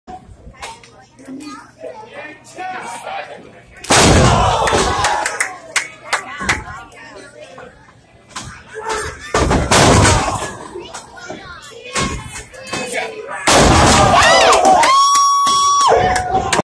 Gory hits the QAS on Facade in BDW 6 Man High Stakes Match!
At the Diamondplex in Benwood WV for Black Diamond Wrestling's 15th Anniversary Show, after DJ Z catches Facade in a facebuster on his knee, Gory leaps over Z to hit the Quick As Shit.